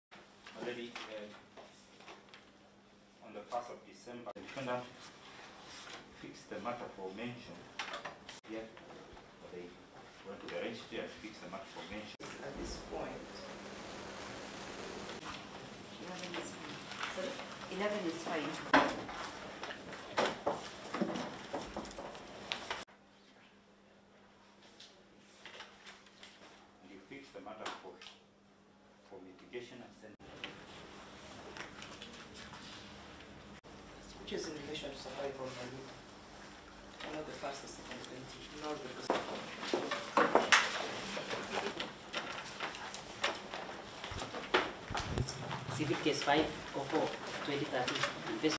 Cyprian Nyakundi jailed for 4 months: Popular Kenyan blogger jailed! Breaking news
Amidst fighting back tears, the controversial blogger, was sentenced to four months in jail by a Nairobi court